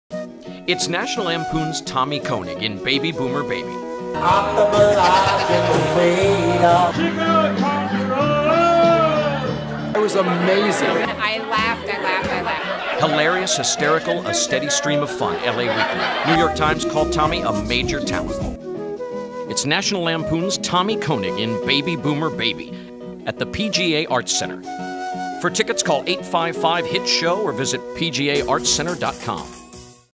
Baby Boomer Baby at the PGA Arts Center
Coming on April 27 to the PGA Arts Center, a very funny show starring Tommy Koenig, a new one-man musicomedy!  Visit PGAArtsCenter.com. Tickets call 855-HIT-SHOW.